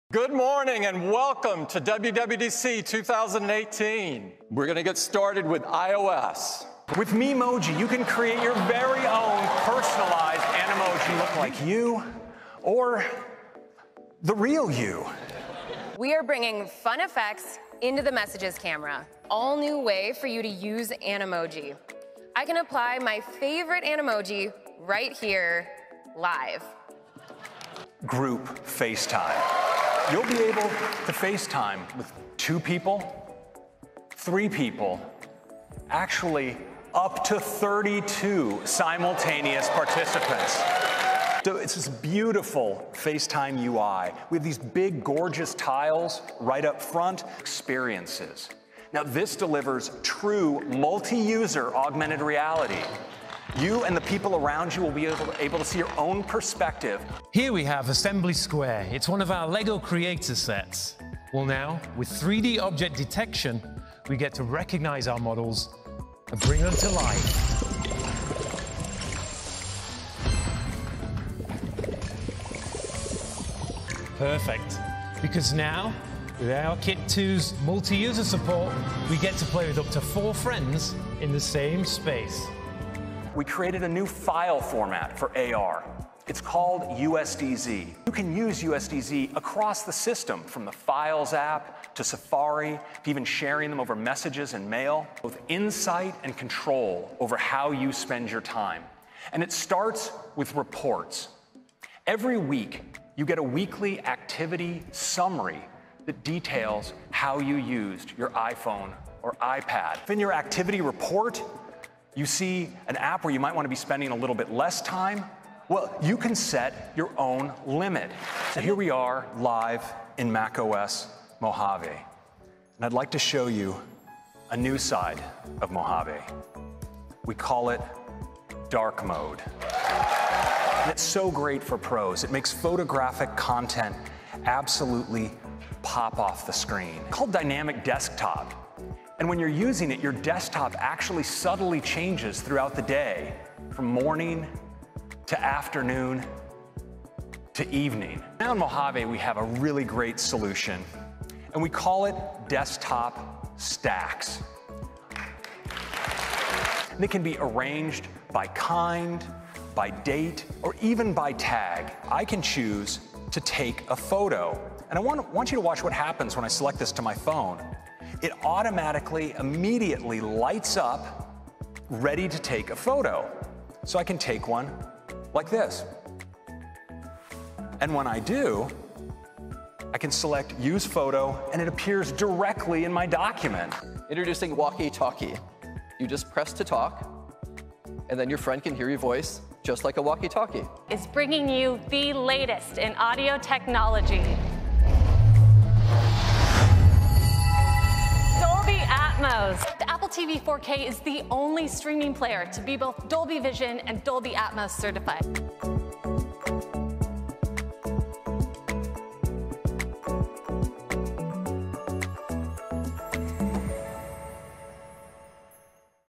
WWDC: 10 new OS features we can't wait to try
While there were no new hardware or jaw-dropping announcements made at this year's Worldwide Developers Conference (WWDC) in San Jose, Apple did introduce some pretty useful features that will make their way into the upcoming software updates for its key devices. Here's our selection of 10 nifty new features we can't wait to try.